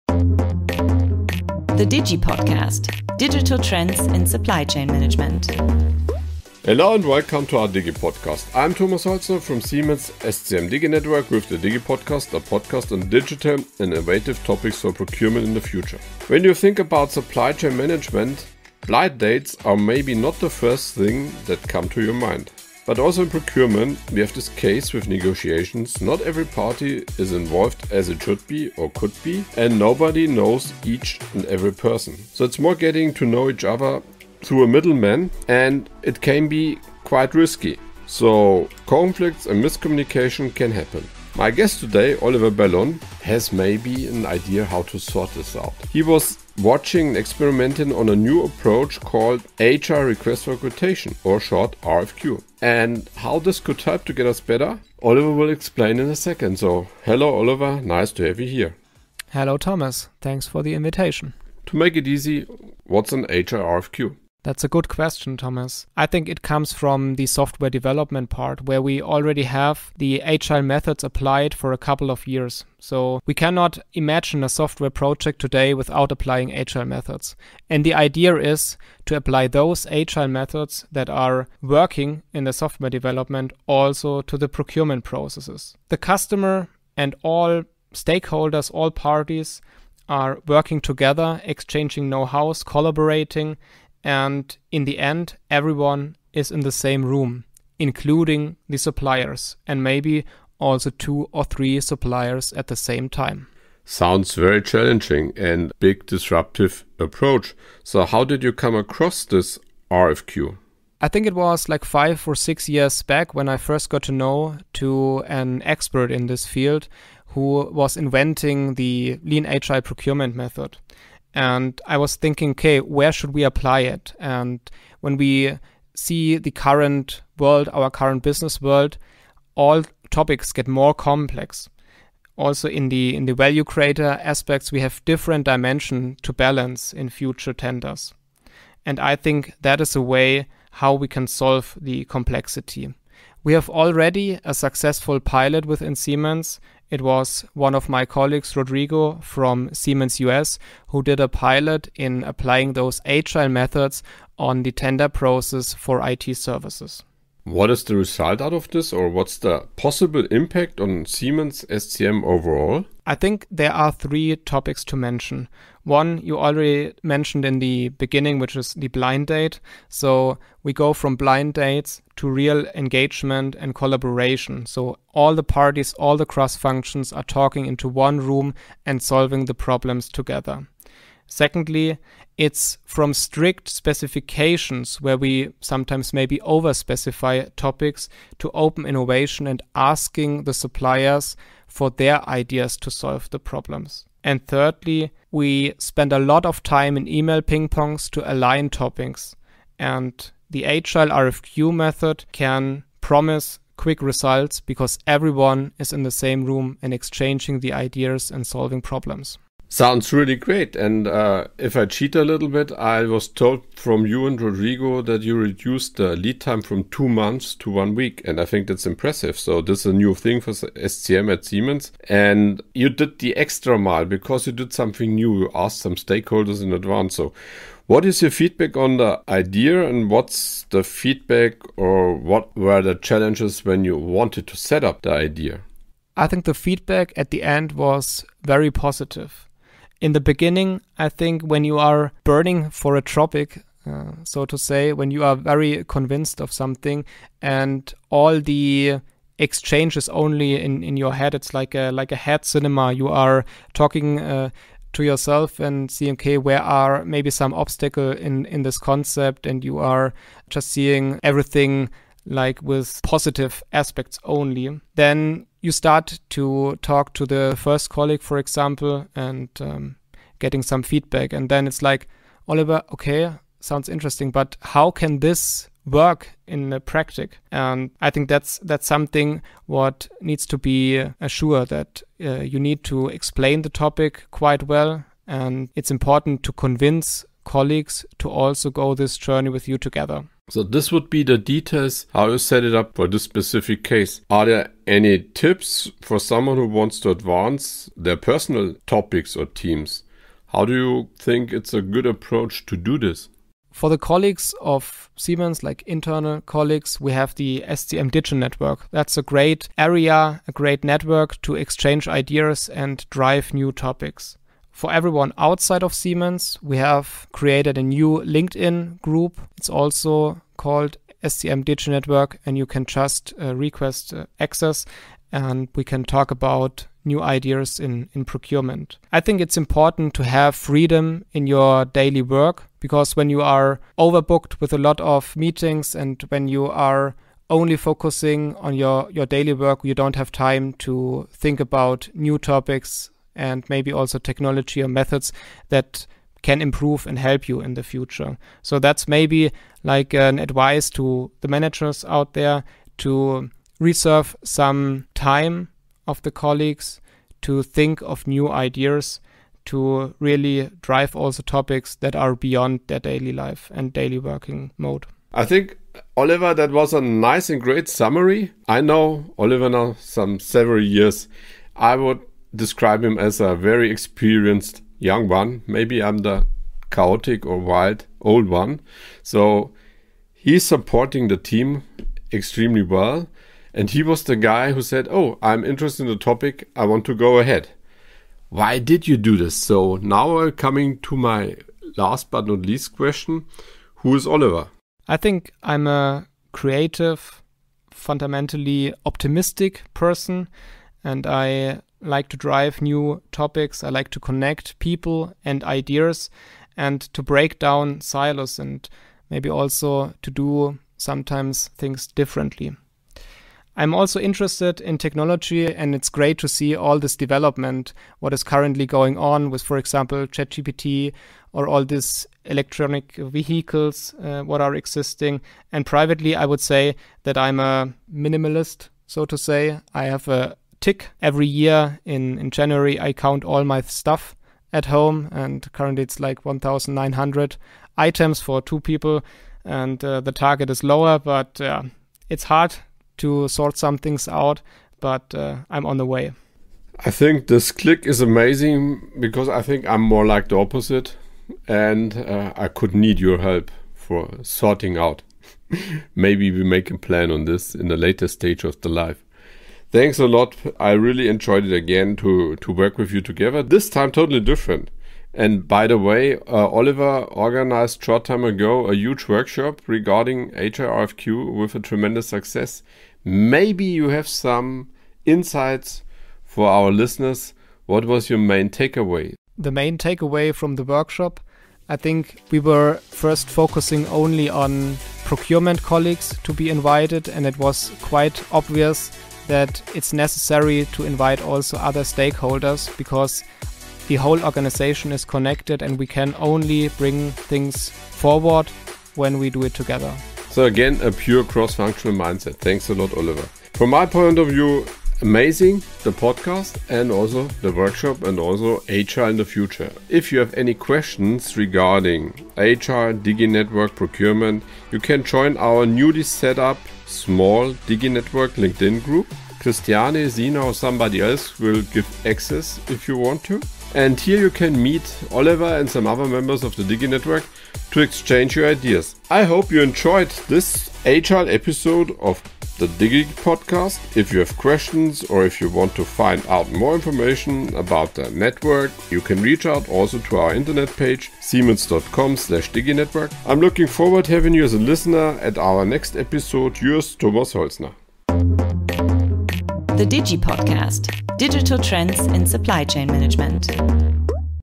#26 DigiPodcast: How SCM could put an end to blind dates
Communication is key, both in private and professional partnerships. Active listening, interaction and agility are indispensable in this context. With the so-called agile request for quotation or agile RfQ, Oliver Bellon has taken an innovative step to make tenders and projects in purchasing more collaborative. This approach brings all partners closer together right from the start and counteracts relationship crises right from the start. The result: more effective collaboration that, above all, saves time and nerves. You can hear exactly how this works in this episode of the DigiPodcast with host Thomas Holzner